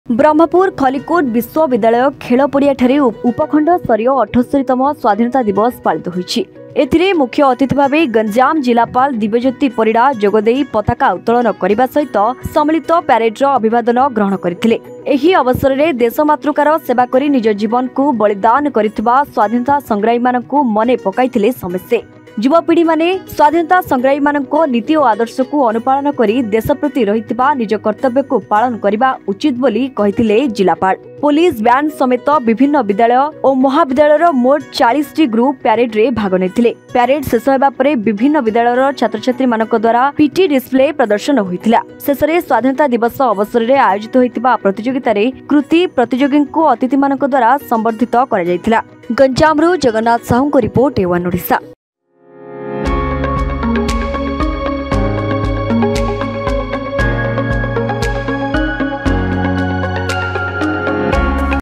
ଖଲ୍ଲିକୋଟ ବିଶ୍ବବିଦ୍ୟାଳୟରେ ସ୍ୱାଧୀନତା ଦିବସରେ ପତାକା ଉତ୍ତୋଳନ କଲେ ଜିଲ୍ଲାପାଳ | GANJAM NEWS
ଖଲ୍ଲିକୋଟ ବିଶ୍ବବିଦ୍ୟାଳୟରେ ସ୍ୱାଧୀନତା ଦିବସରେ ପତାକା ଉତ୍ତୋଳନ କଲେ ଜିଲ୍ଲାପାଳ | @a1odisha